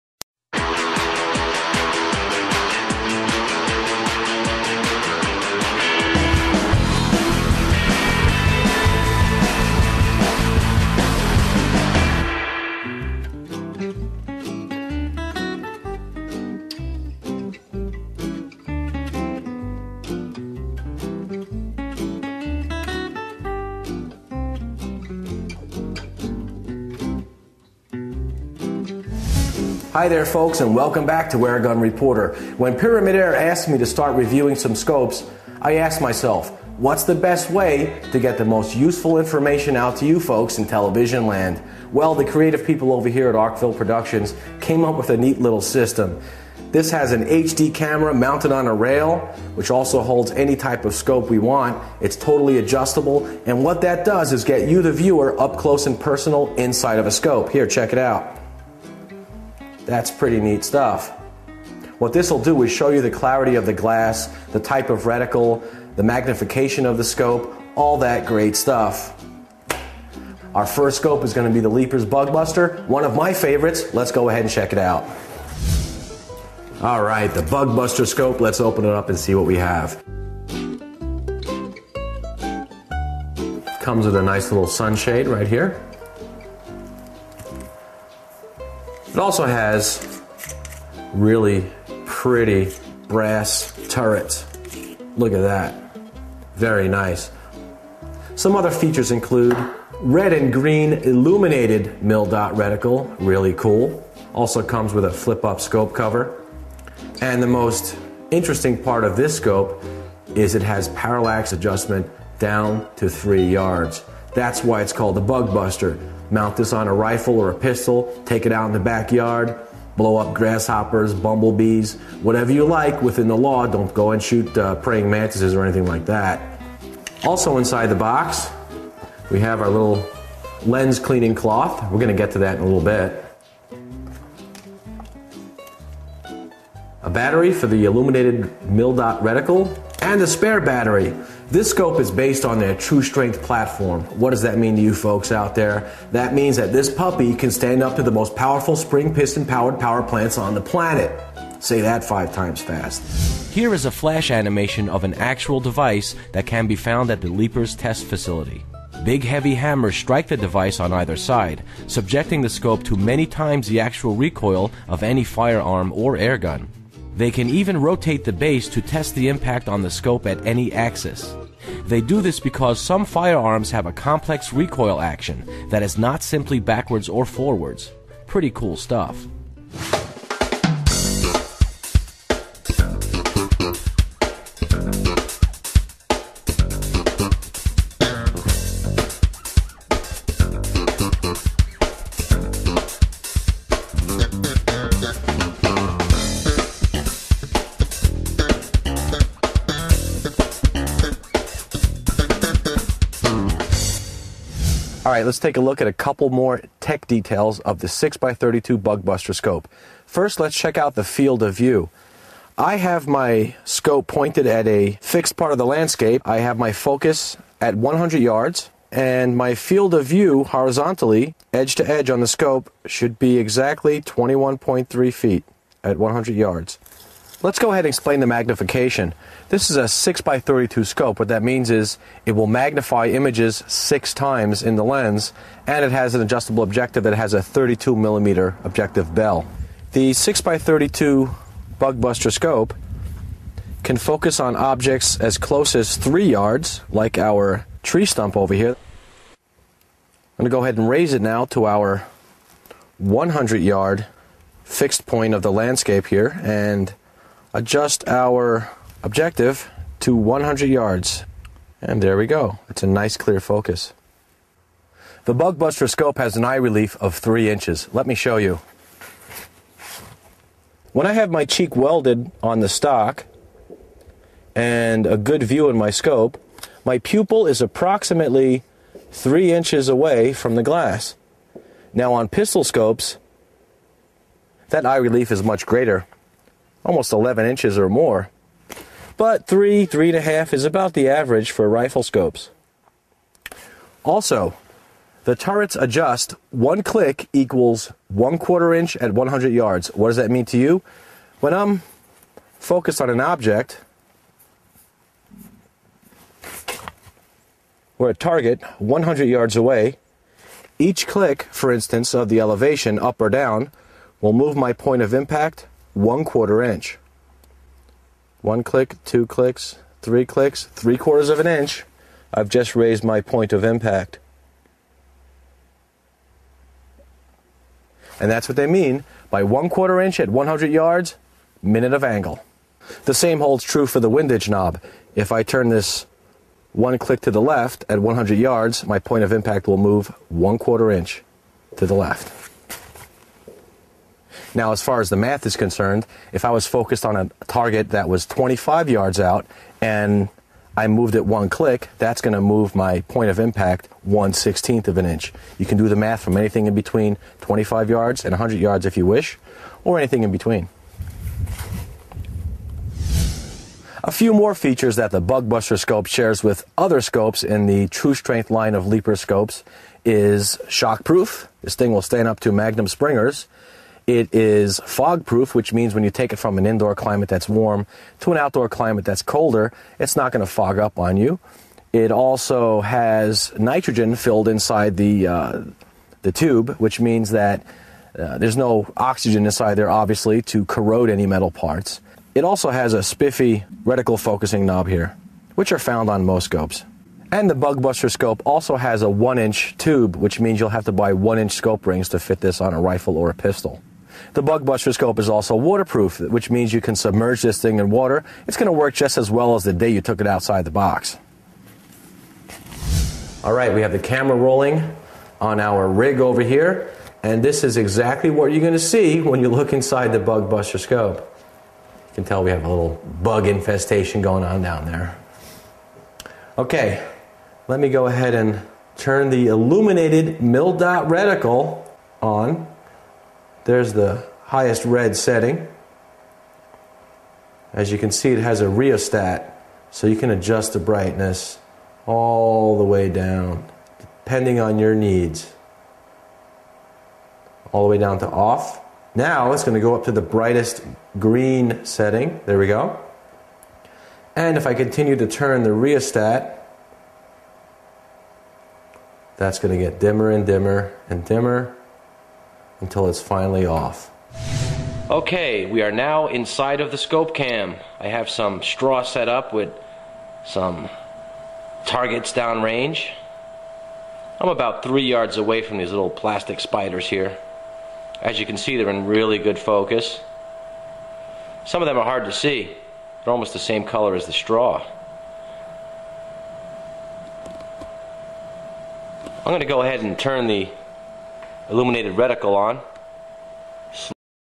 Bug Buster Scope - AGR Episode #26
Paul reviews Leapers Bug Buster scope.